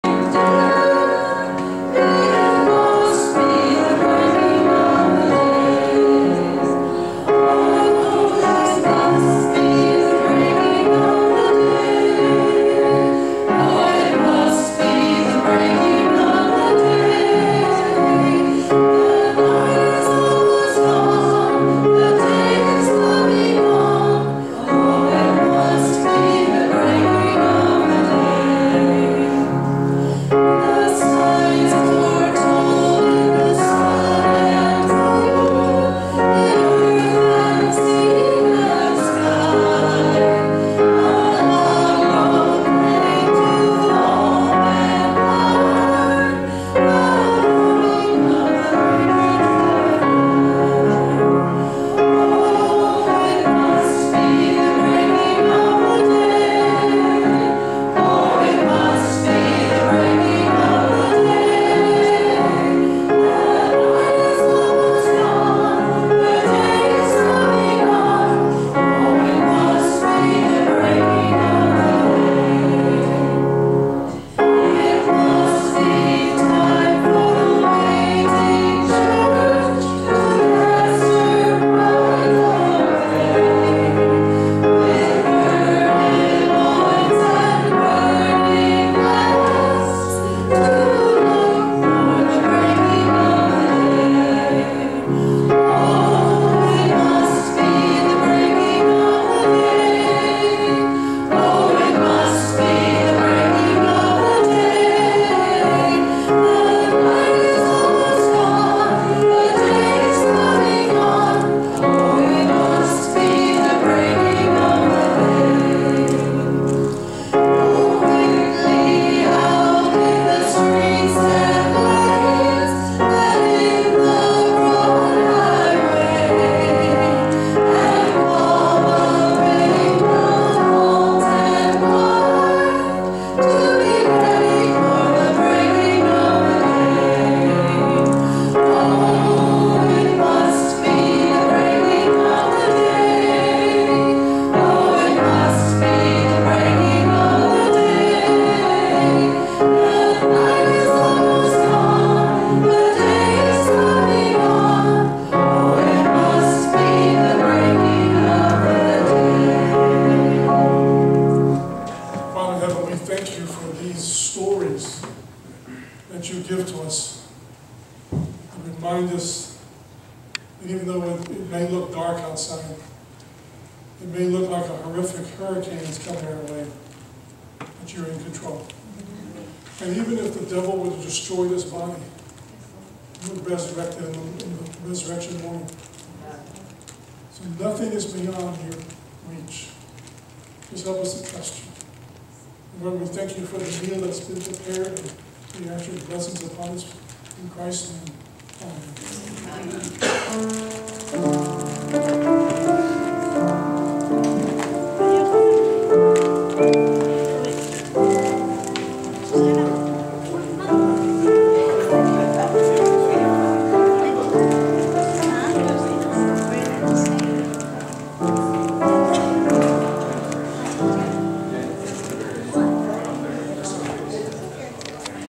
NSBSDA - Sermon Part II - September 7, 2019
Presented By: Dennis
Sermon Title: Daemon: God of Second Chances
New Smyrna Beach Seventh Day Adventist Church